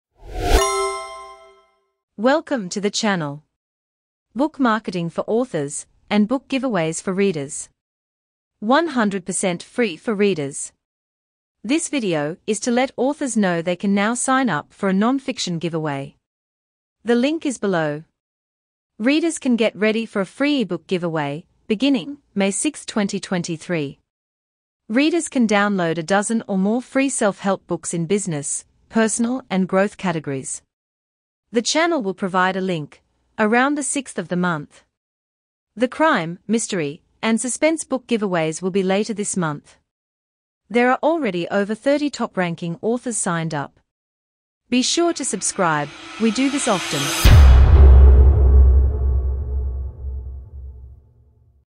For Authors Signup for Giveaway Readers Coming Soon Non-Fiction Giveaway
1.  Bookfunnel.com

2.  Platform to collect your emails FREE for the first 1000 signups
This is a new site that thousands of authors are using

Taking action on this will ensure you new readers and more book sales.

This video is to let authors know they can now sign up for a non-fiction giveaway. The link is below.

The Crime, Mystery, and Suspense Book Giveaways will be later this month. There are already over 30 top-ranking authors signed up.